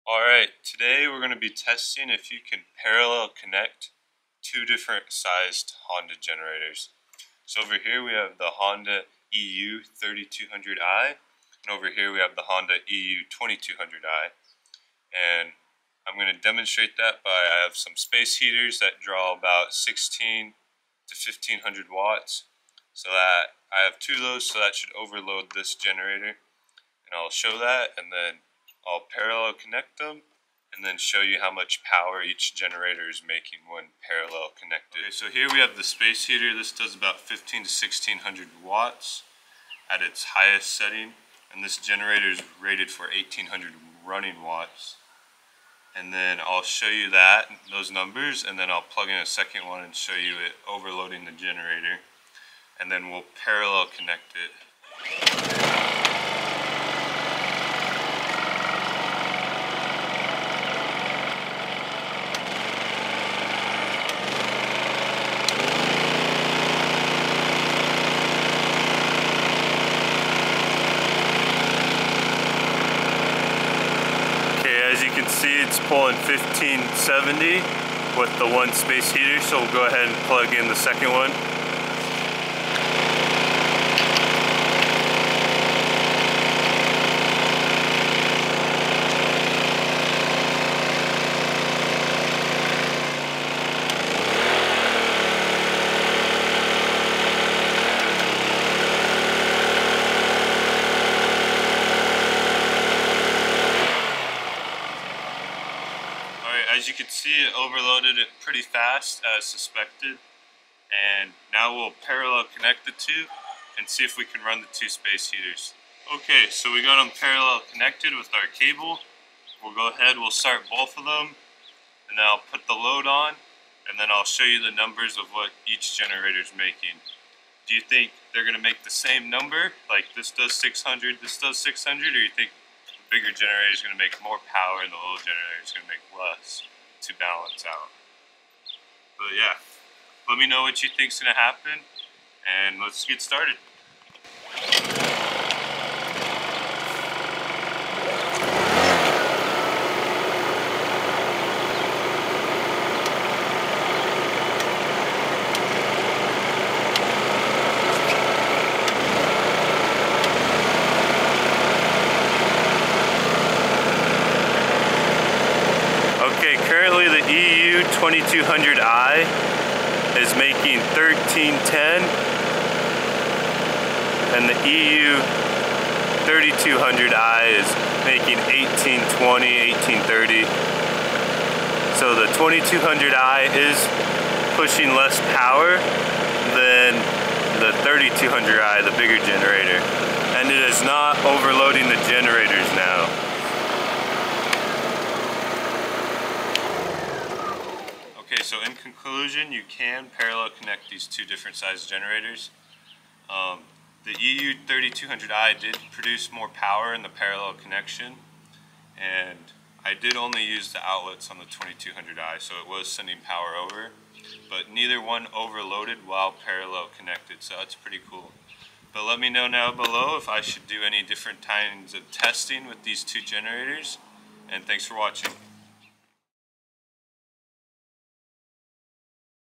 Can you parallel connect these two different generators? EU3200i & EU2200i
In this video I test if you can parallel connect the Honda EU3200i generator with the EU2200i generator and I show what the power output is between the two when connected.